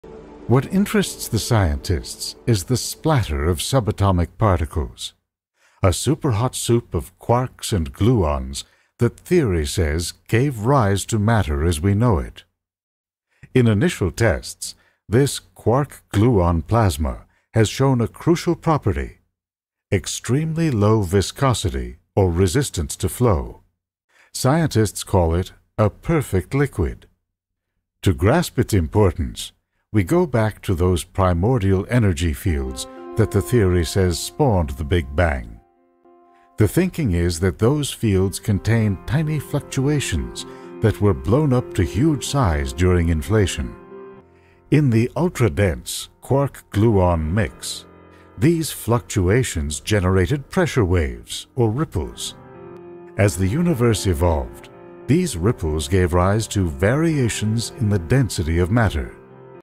🌌 Unveiling the Universe's Primordial Soup! 🔥
Ever wondered what the universe was like in its earliest moments? Scientists call it the primordial soup—a hot, dense mix of particles that eventually formed everything we see today! 🌠

🚀 Join us as we break down the origins of the cosmos in just 60 seconds!